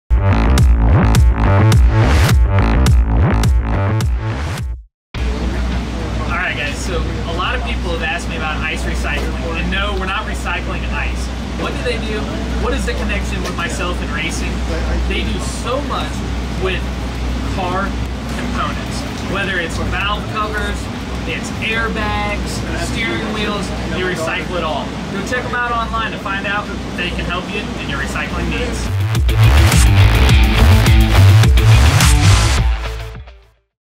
Racing and Recycling with Spencer Boyd
Spencer Boyd talks racing and recycling.